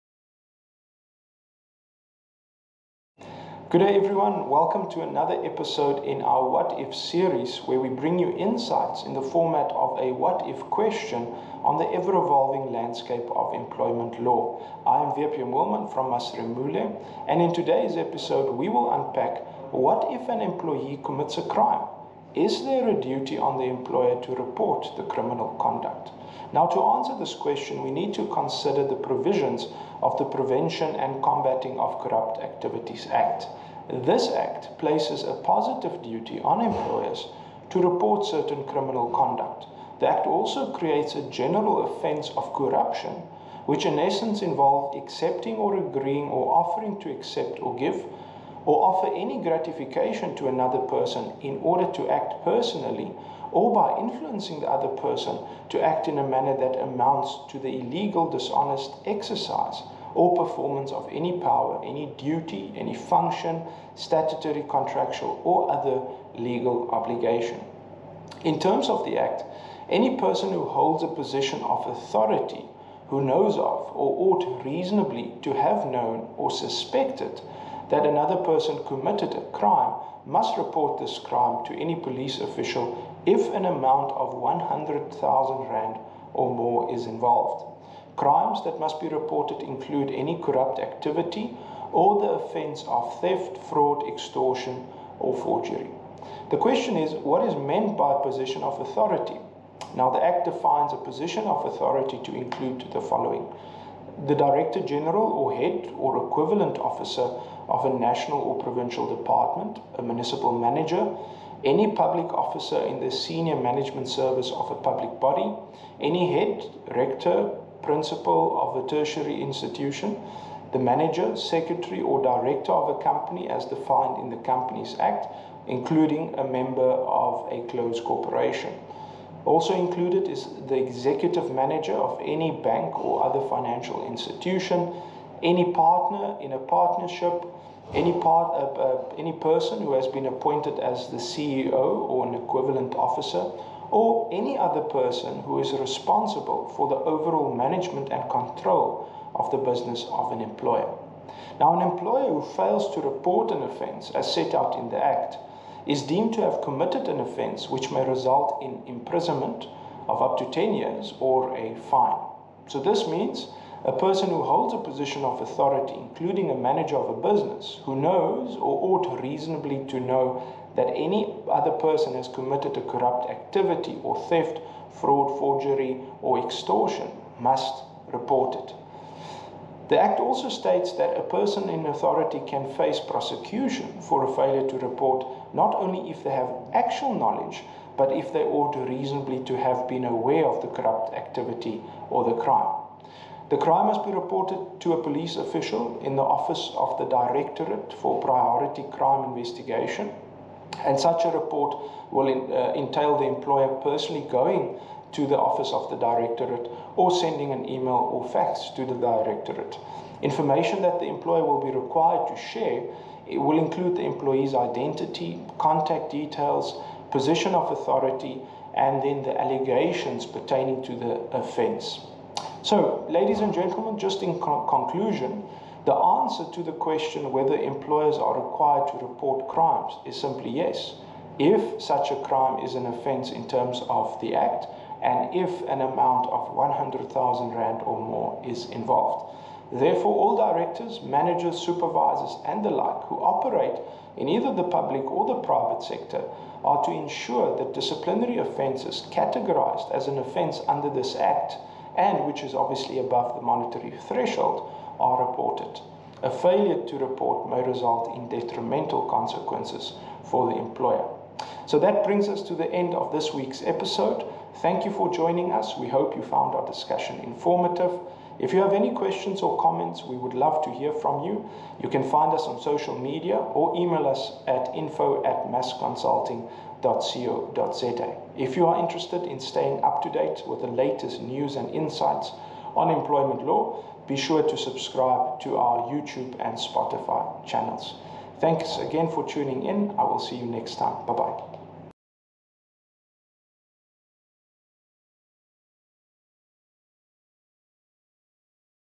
What if an employee commits a crime? Is there a duty on the employer to report the criminal conduct?
For more articles and corporate employment law resources visit our